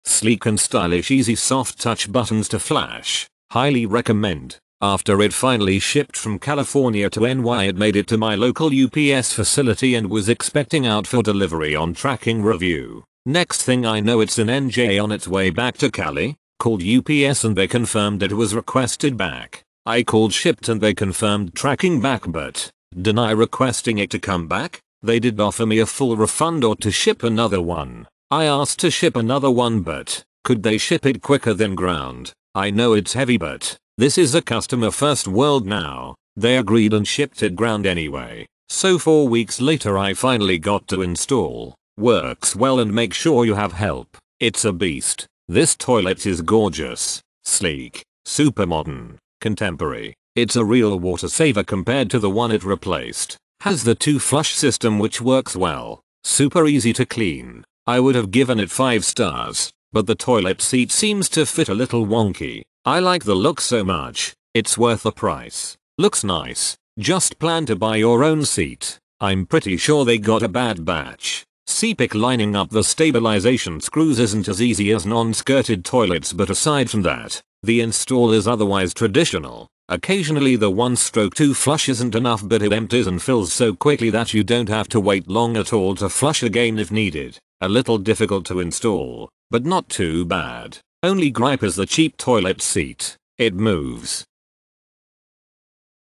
DeerValley DV-1F52636 Modern Comfortable Seat Height Dual Flush Elongated One-Piece review
DeerValley DV-1F52636 Modern Comfortable Seat Height Dual Flush Elongated One-Piece Toilet with Soft Closing Seat, High-Efficiency Supply, Luxury White Contemporary Ceramic (White)

Main Features:
-  [Comfort Design]: Features approximately 17" height from floor to seat. Chair-height seating, it makes visiting the toilet to be more comfortable as there is less straining sitting on or standing up. It is ideal for tall adults, the elderly, and the disabled.
- [Easy to Clean Design ]: The glazed surface skirted trap way creates a sleek look and makes cleaning easier. Compare to other toilets, it has no corners and grooves, very easy to reach for cleaning.
- [Siphon Flushing]: Quite & Powerful Flushing one piece toilet, Fully glazed flush system, Highly efficient dual-flush bringing a super quiet and powerful flushing - NO clogs, NO leaks, and NO problem.
- [Easy Install]: The toilet is easily installed, it can more Convenient. Two Sizes of Supply Line Inlet Hose, this toilet will come with a supply line connector (adapter) for you to connect the supply line and toilet. If your supply line size is 7/8", you could directly connect your water line to the toilet. If your supply line size is 1/2", please screw off the adapter, then connect it.
-  [All-In-One package]: Package Includes toilet, pre-installed soft closing toilet seat(Seat Material: plastic), pre-installed flush valve, high-quality wax ring, floor bolts, and also installation instruction.
- [(Warranty)] : DeerValley toilets are warranted to be free of defects for one year from the date. One-year Limited warranty on flushing mechanism and Soft-Closing toilet seat lid. Lifetime after-sale service, 24 hours online.